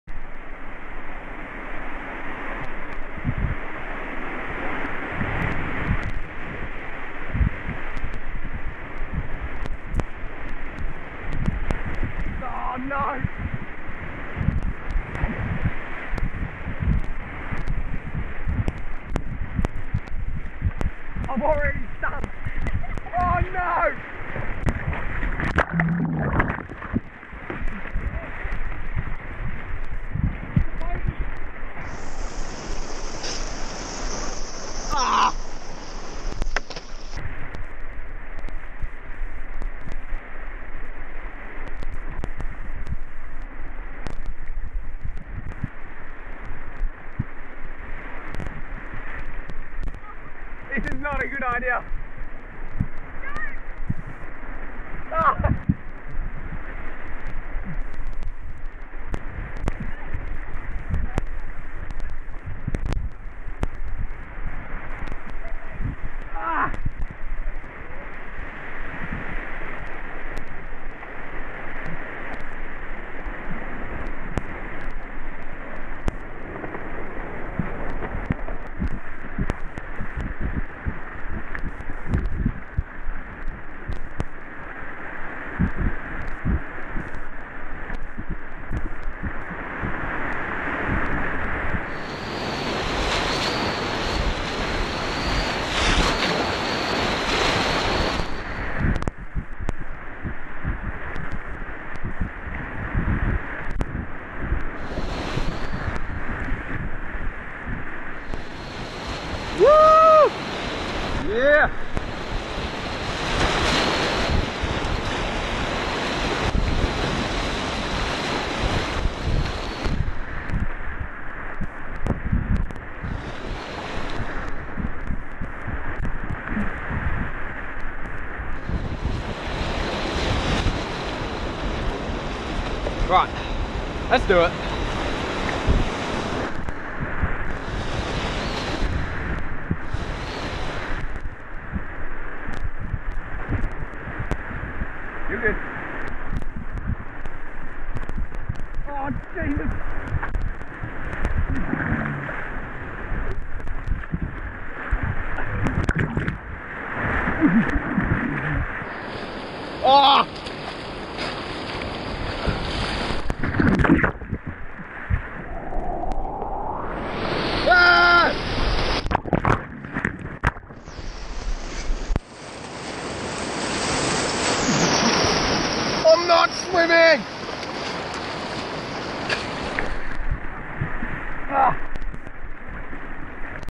Kayaking with no spray deck
Kayaking down Penrith White Water Stadium in Sydney with no spray decks.

This is a raw edit.
Shot on a GoPro Hero 4 Session,
Shot in 1080p in 'super view' at 48 fps